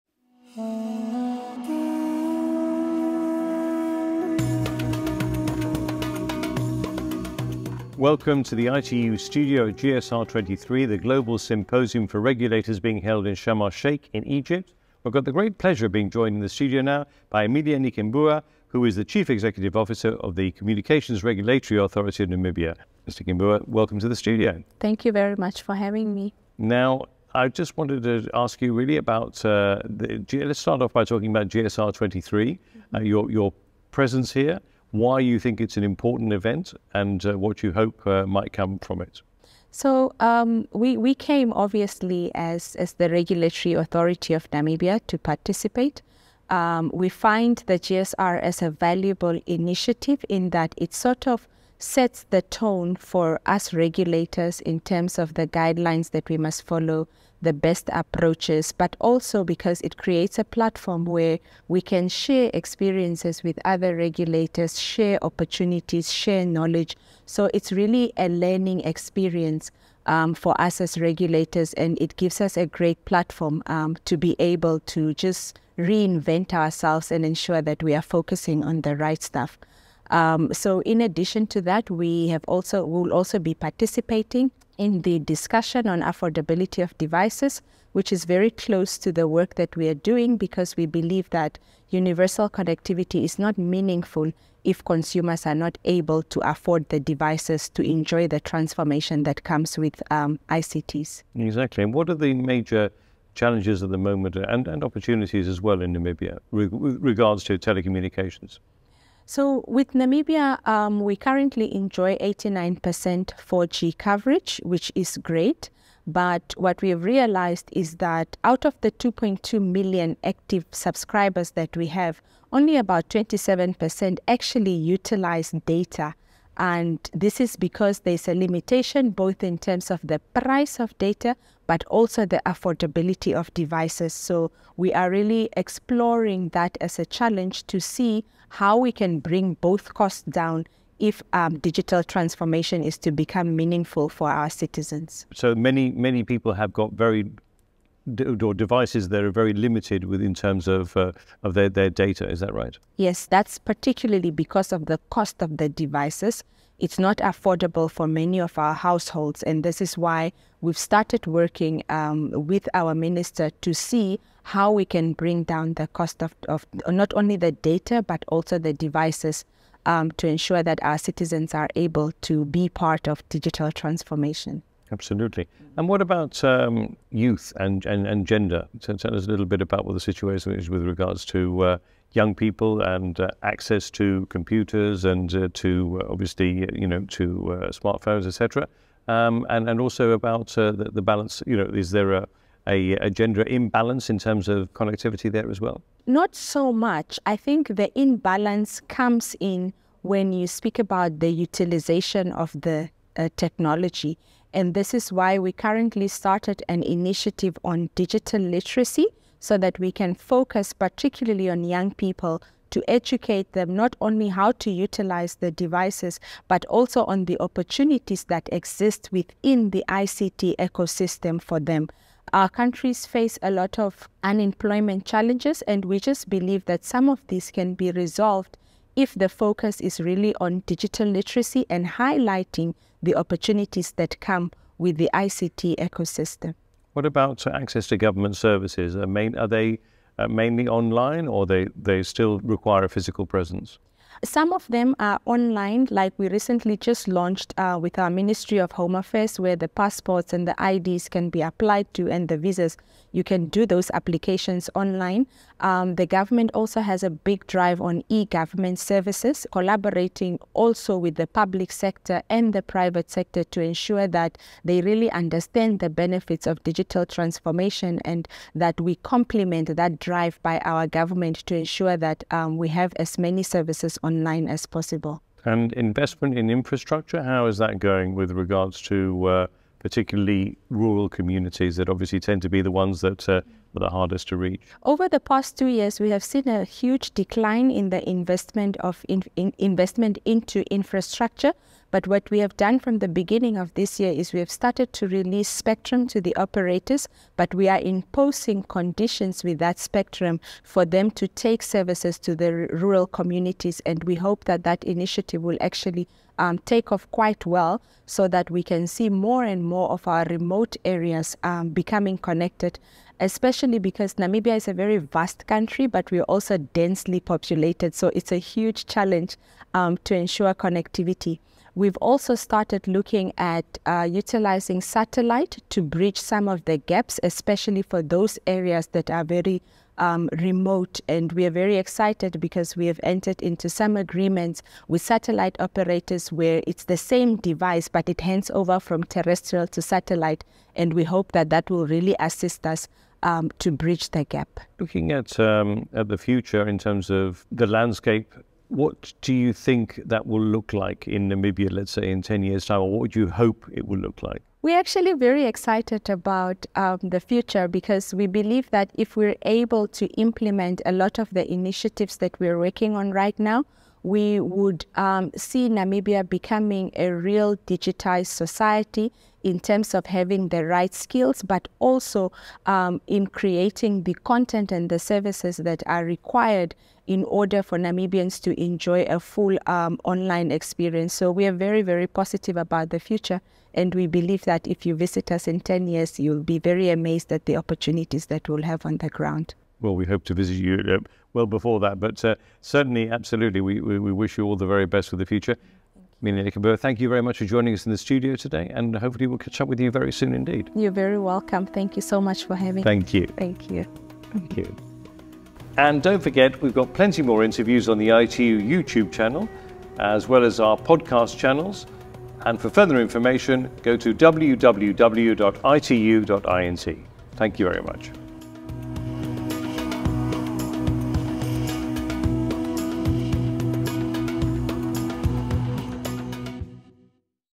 ITU INTERVIEWS @ GSR-23:Emilia Nghikembua, CEO, Communications Regulatory Authority, Namibia
Interview with Emilia Nghikembua, Chief Executive Officer, Communications Regulatory Authority, Namibia, at the Global Symposium for Regulators (GSR-23) 2023, Sharm El-Sheikh, Egypt.